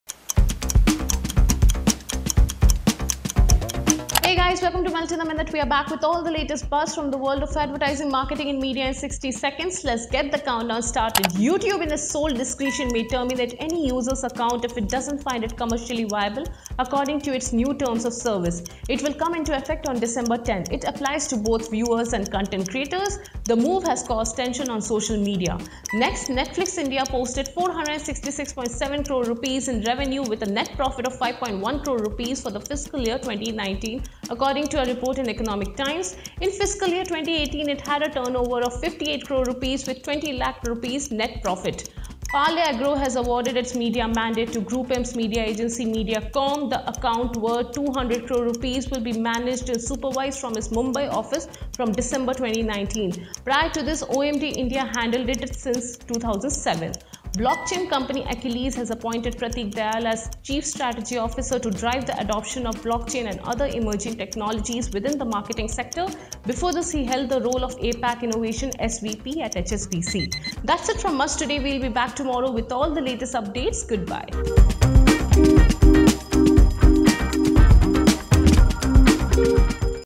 Melt In A Minute | YouTube can delete your account if it isn't 'commercially viable' & More
Today's Top News:
1. YouTube can delete your account if it's isn't 'commercially viable'
2. Netflix India posted Rs 466.7 crore in revenue in FY19
3. Parle Agro awards its media mandate to Mediacom
4. Aqilliz has appointed Prateek Dayal as Chief Strategy Officer